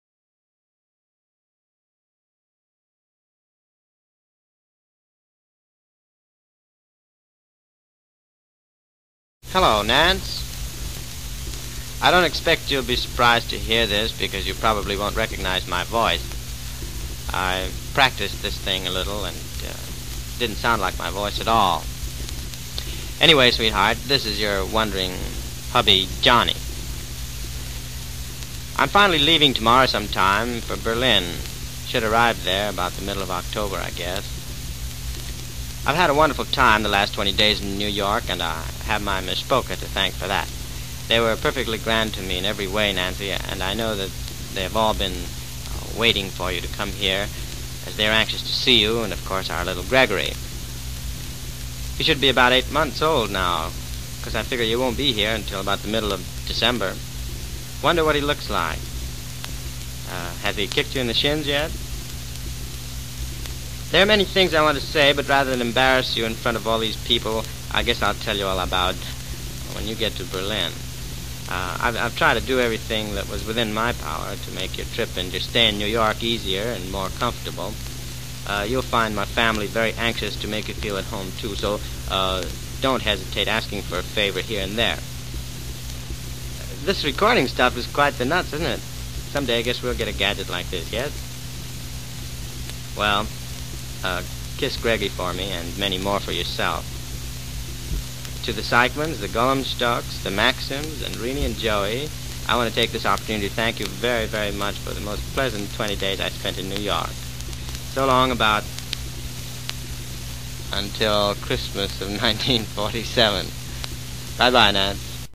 Home Recorded Tru-Kut Disc Recorded Letter from G.I. to Wife 1947 on way to Berlin
This is a spoken letter from a G.I. in the Army to his wife in the States from an old Tru-Kut home recorded disc.   Interesting that he's on his way to Berlin and he found someplace to make this record.